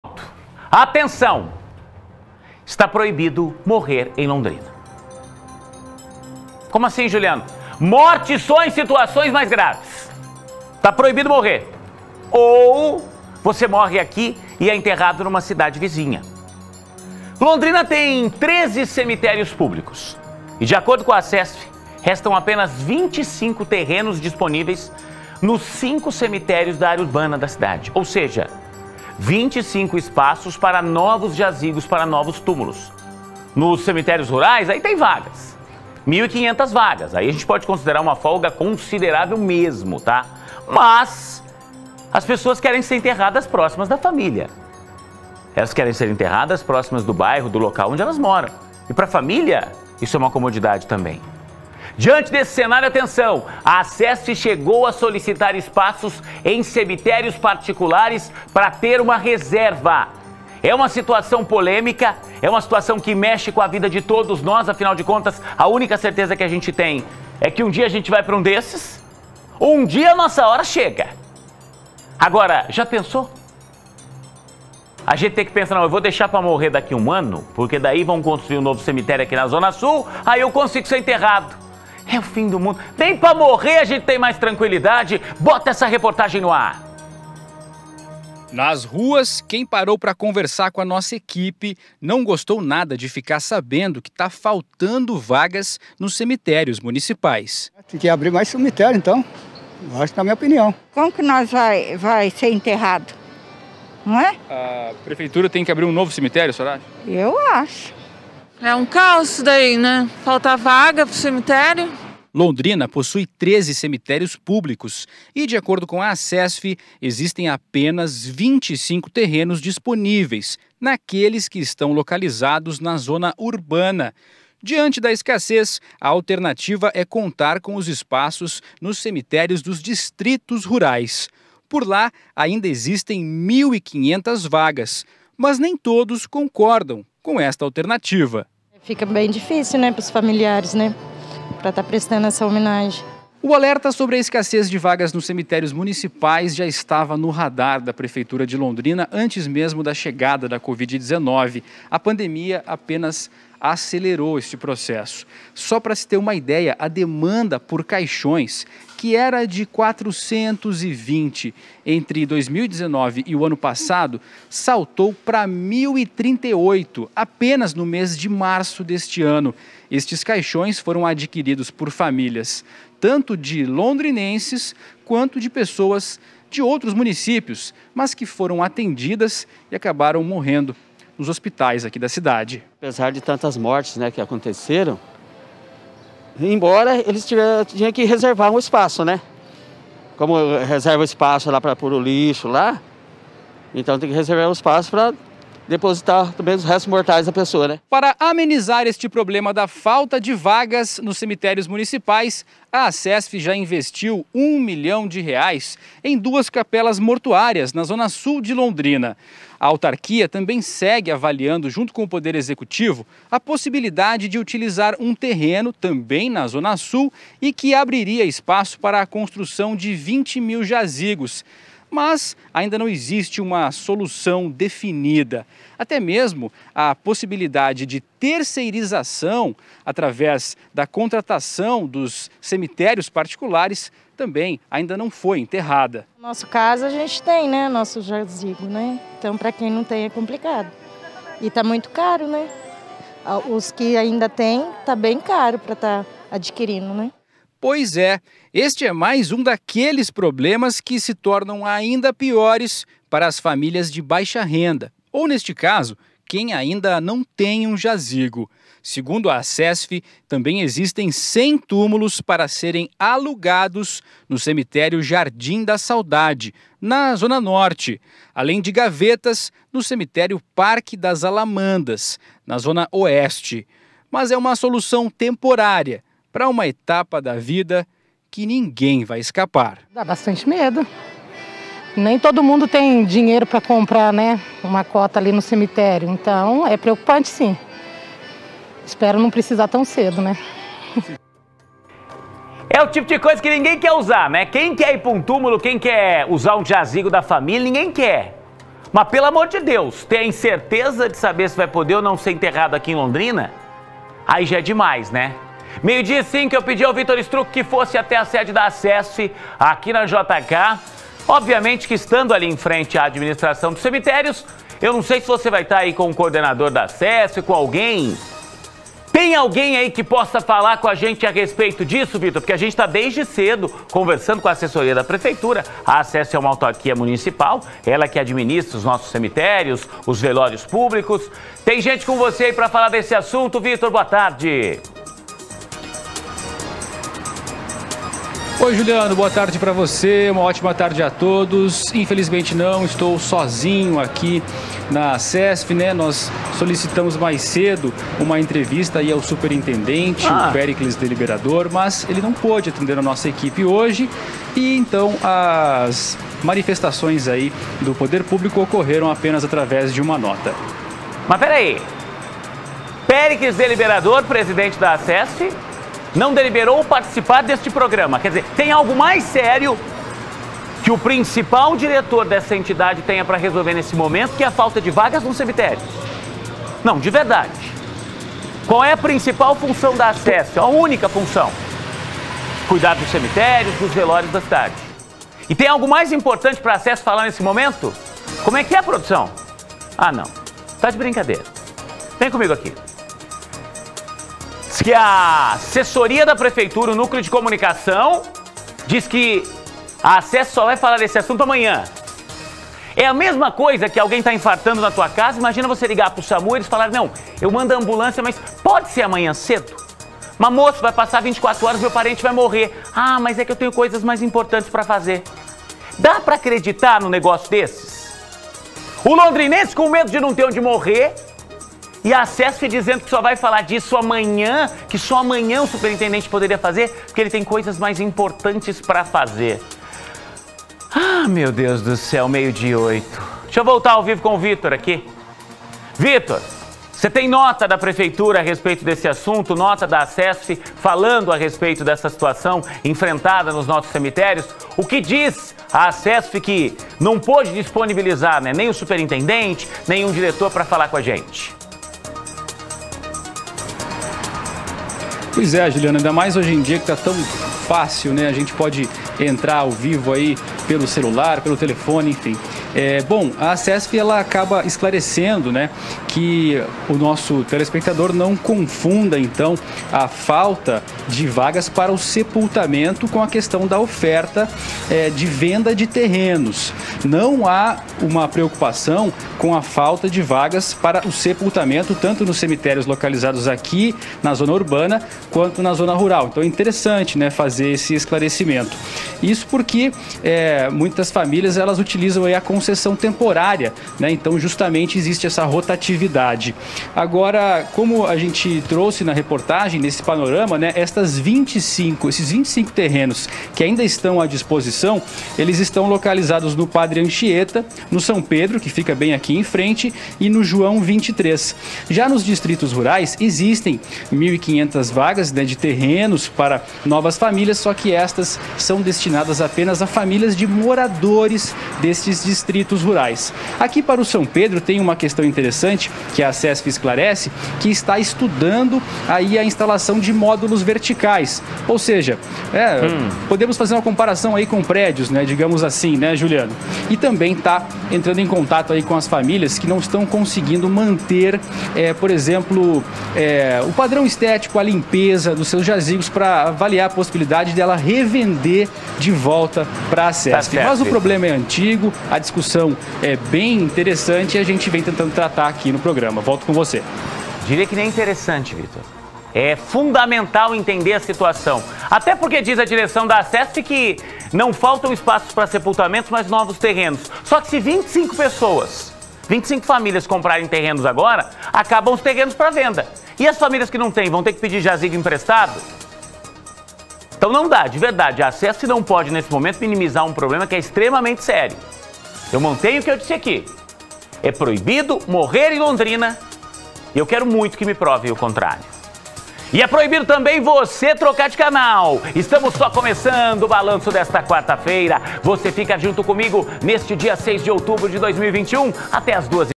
Vagas nos cemitérios urbanos perto do fim: novos túmulos serão na Zona Rural
Polêmica dos cemitérios, Londrina tem 13 cemitérios públicos e, de acordo com a Acesf, restam apenas 25 terrenos disponíveis nos cinco cemitérios da área urbana,  ou seja, 25 espaços para novos túmulos, nos cemitérios rurais, há 1.500 vagas, ou seja, uma folga considerável, mas nem todo mundo gostaria de ter o jazigo da sua família distante da cidade, diante deste cenário, a Acesf chegou a solicitar espaços em cemitérios particulares para ter como reserva.